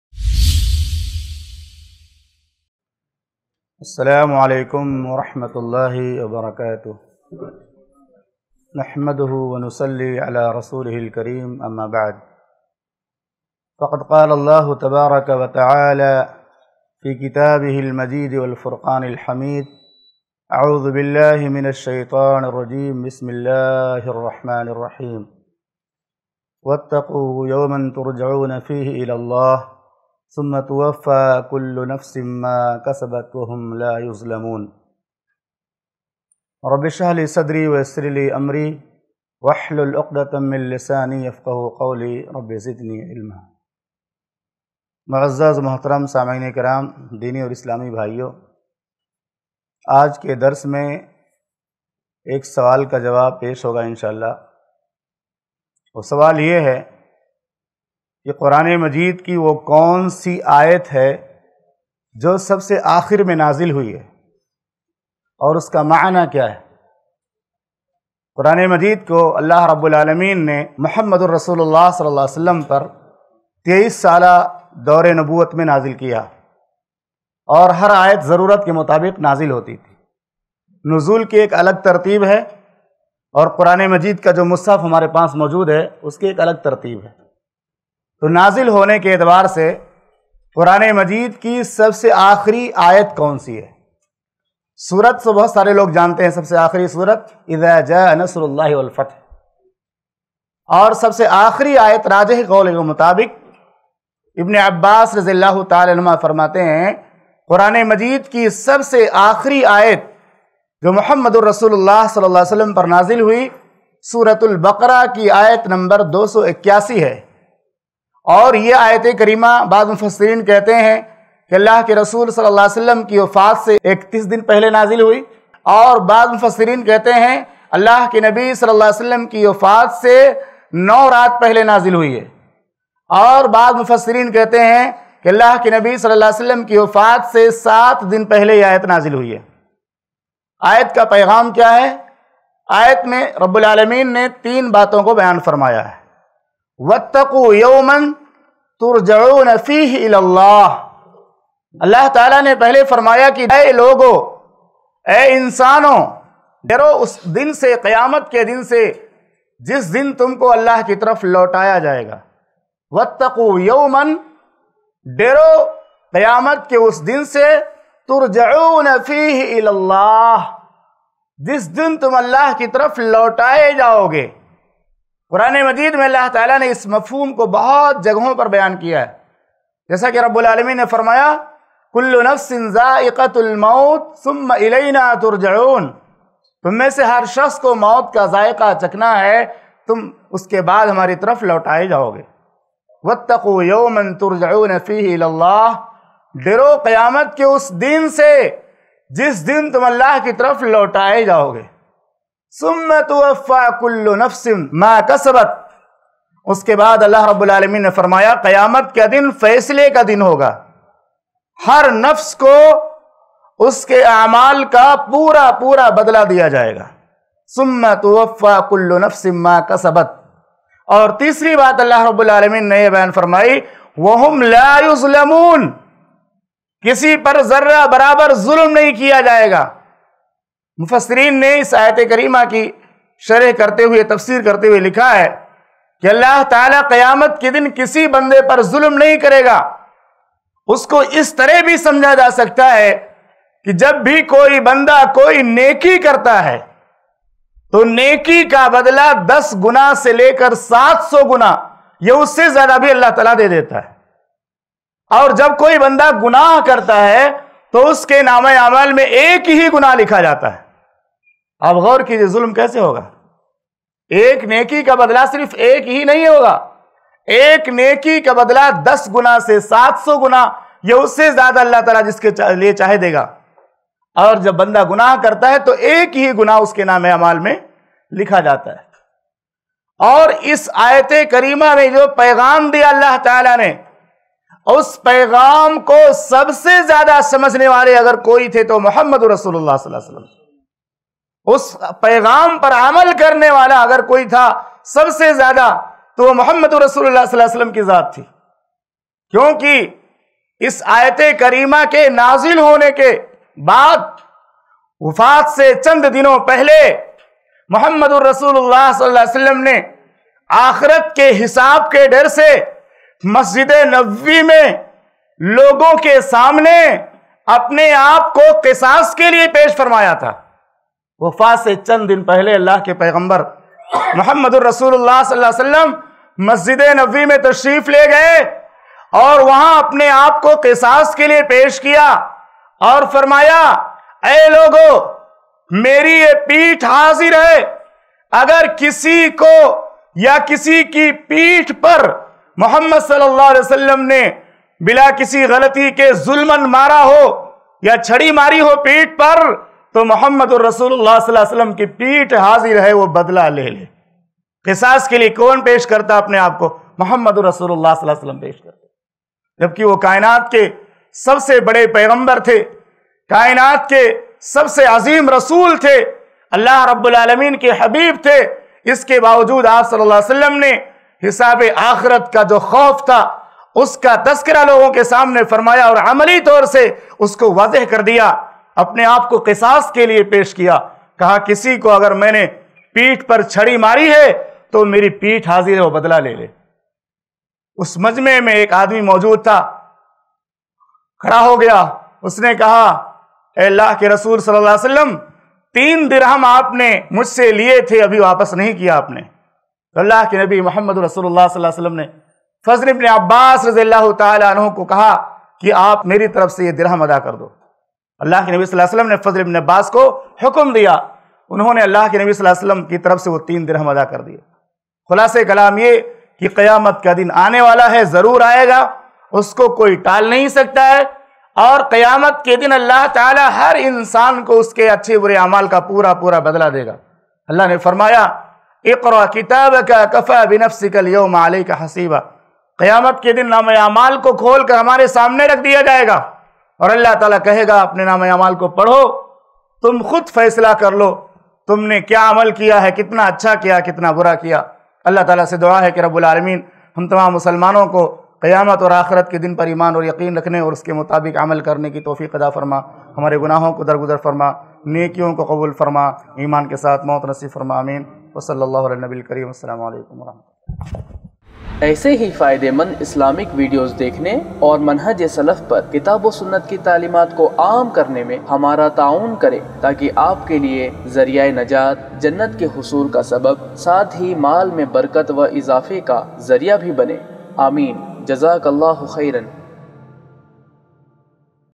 Quran Ki Kaun Si Aayat Sab Se Aakhir Mein Nazil Huyi ? By Shaikh Abul Bayan Salafi | IIC Mumbai
Bank Account Details:
ILM FOUNDATION
ICICI Bank
Savings account
IFSC CODE-ICIC0001028

JazakAllah Khair